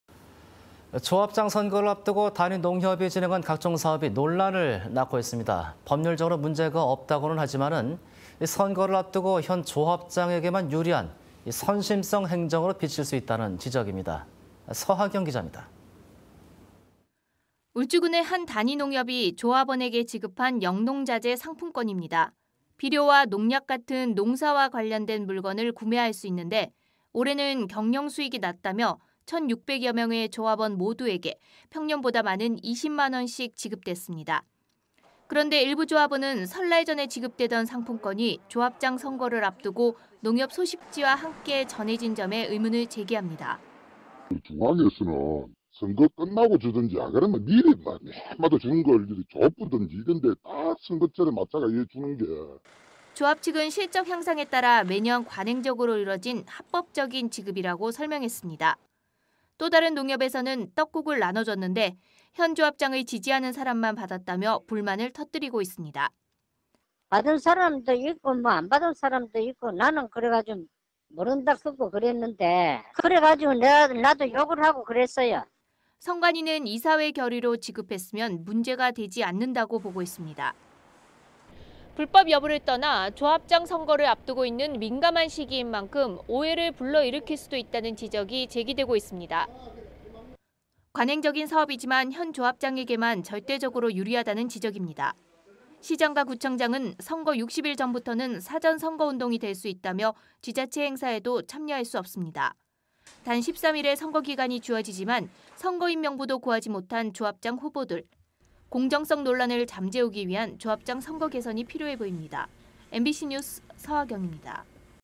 조합장 선거 기울어진 운동장
조합장 선거를 앞두고 단위 농협이 진행한
각종 사업이 논란을 낳고 있습니다.

법률적으로 문제가 없다고 하지만
선거를 앞두고 현 조합장에게만 유리한
선심성 행정으로 비칠 수 있다는 지적입니다.

울산MBC는 여러분의 소중한 제보를 기다립니다.
울산MBC 카카오톡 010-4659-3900